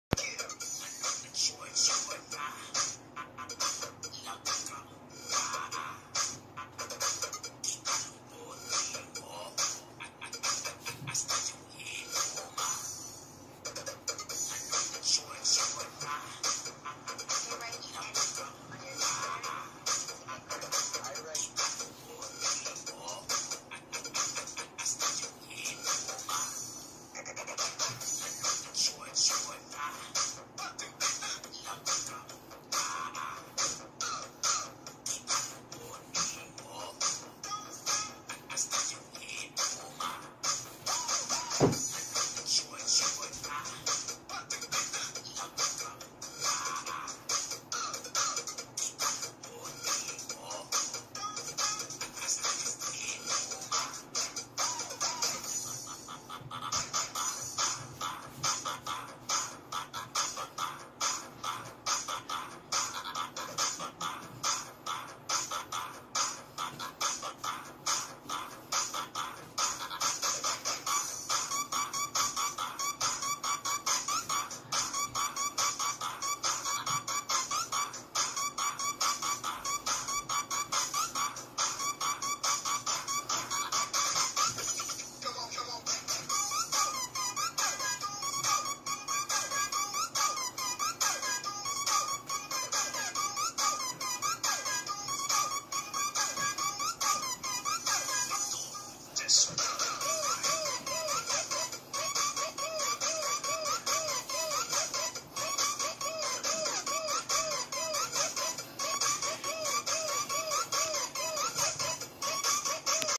BUDOTZ Dagohoys version - Resulta ng lockdown
Despite of all difficulties and challenges we are currently facing, we as human beings are  capable of entertaining ourselves just to coup up and forget the miseries, hardships that  we experienced in this world.  Kaya mga ka dagohoy, sayaw muna tayu ng budotz para masaya.

(Late upload)